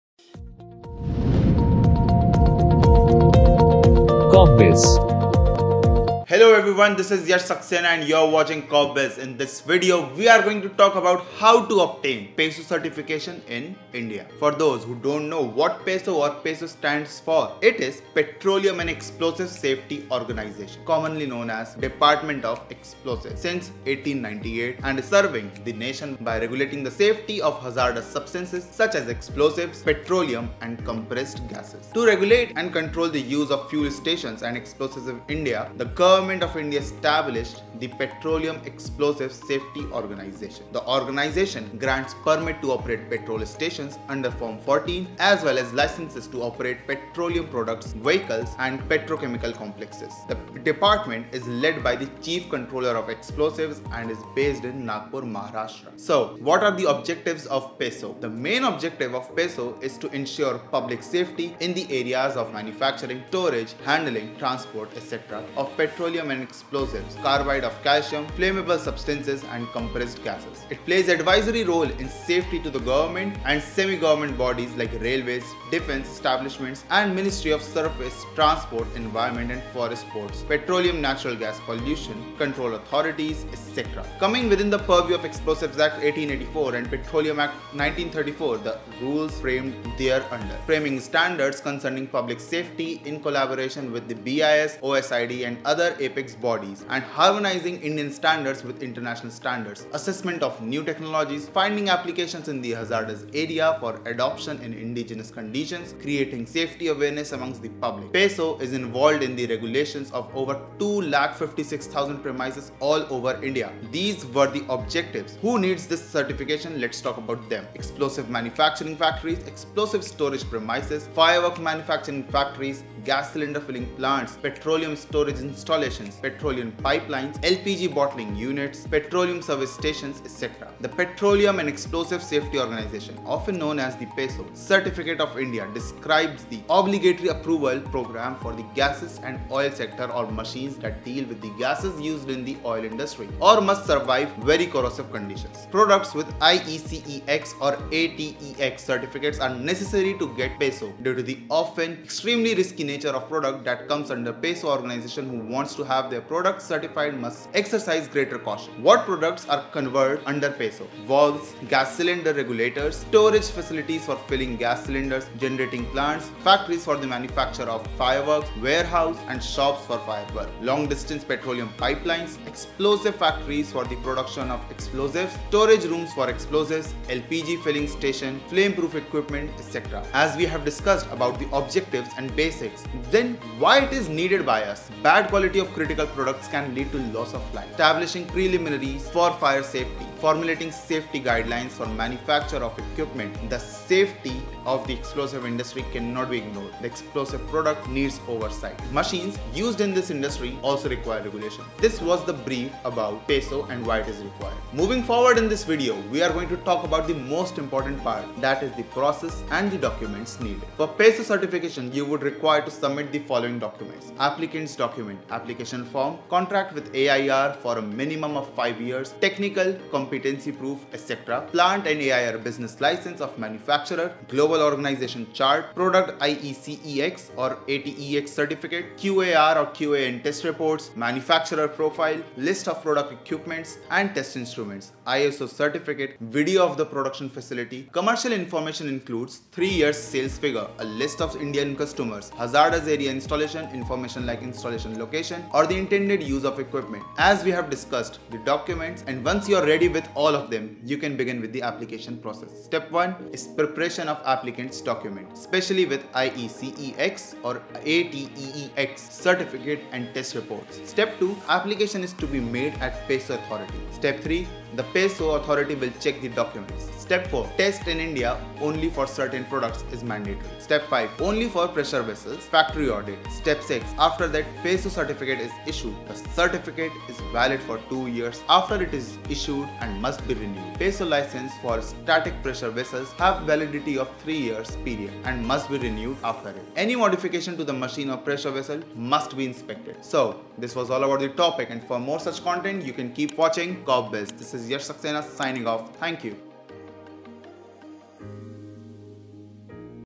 How to Obtain PESO Certification in India? | Petroleum and Explosives Safety Organization | Corpbiz
For those who don’t know what PESO or peso stands for it is Petroleum and Explosives Safety Organization, commonly known as the department of Explosives, since 1898 and is serving the nation by regulating the safety of hazardous substances such as explosives, petroleum, and compressed gas.

To regulate and control the use of fuel stations and explosives in India, the government of India established the Petroleum Explosives Safety Organisation. The organization grants permit to operate Petrol Stations under Form 14, as well as licenses to operate Petroleum Products Vehicles and Petrochemical Complexes. The department is led by the Chief Controller of Explosives and is based in Nagpur, Maharashtra, India.

What are the objectives of PESO-

• The main objective of PESO is Ensure public safety in the areas of manufacturing, storage, handling, transport, etc., of petroleum and explosives, carbide of calcium, flammable substances and compressed gases.
• Advisory role in safety to the government and semi-government bodies like railways, defence establishment & Ministry of Surface Transport, Environment &Forest  Ports, Petroleum Natural Gas, Pollution Control Authorities etc. coming within the purview of Explosives Act 1884 and Petroleum Act 1934 and the rules framed thereunder.
• Framing standards concerning public safety in collaboration with BIS, OSID and other apex bodies and harmonizing Indian standards with international standards.

These were the objective, who needs this certification…
• Explosives Manufacturing Factories
• Explosives storage premises
• Firework manufacturing factories
• Gas Cylinder filling plants
• Petroleum Storage installations
• Petroleum Pipelines
• L.P.G. bottling units
• Petroleum service stations

The Petroleum and Explosive Safety Organisation, often known as the PESO certificate of India, describes the obligatory approval program for the gas and oil sector or machines that deal with gases used in the oil industry or must survive very corrosive conditions. Products with IECEx or ATEX certificates are necessary to get PESO.

What products are covered under PESO?

• Valves/Gas cylinders/regulators
• Storage facilities for filing gas cylinders
• Generating Plants
• Factories for the manufacture of fireworks
• Warehouse and shops for fireworks
• Long-distance petroleum pipelines
• Explosives
• Factories for the production of Explosives
• Storage rooms for explosives
• L.P.G. filling stations
• Flameproof equipment
As we have discussed the objectives and everything why is it needed?
• Bad quality of critical products can lead to loss of life
• Establishing preliminaries for fire safety
• Formulating safety guidelines for manufacture of equipment
• The safety of the explosive industry cannot be ignored
• The explosive product needs oversight
• Machines used for the industry also require chemical industry require regulations

The most important part that is the process and the documents needed…
For peso certification you would be required to submit the following documents-
• Applicant documents – application form, contract with A.I.R. for a minimum of five years, technical competency proof etc.
• Plant and A.I.R., Business License of Manufacturer
• Global Organisation Chart
• Product IECEx/ATEX certificates
• QAR/QAN Test Report
• Manufacturer Profile
• List of production equipment and test instruments
• I.S.O. certificate
• Video of the Production facility
• Commercial information includes three years’ sales figures, a list of India Customers, hazardous area installation information like installation location or the intended use of equipment

The application process

• STEP 1: Preparation of the Applicant’s Documents, especially with the IECEx/ATEX certificate and test report.
• STEP 2: Application is to be made at PESO Authority
• STEP 3: The PESO Authority will check the Documents
• STEP 4: Test In India only for certain products
• STEP 5: Only for pressure vessels – Factory Audit
• STEP 6: After that PESO certificate is issued

The certificate is valid for two years after it is issued and must be renewed. PESO licenses for static pressure vessels have a three-year validity period and must be renewed.